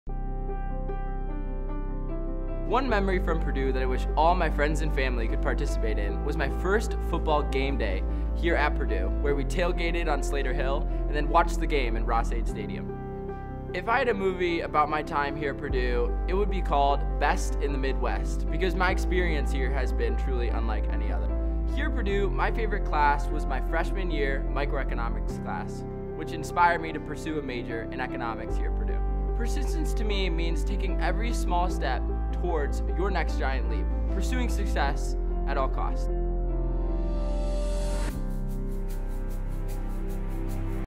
Purdue Students: Meet John Henry
Meet John Henry, a junior majoring in Economics and Communication! Find out what his favorite class is here at Purdue and what persistence means to him.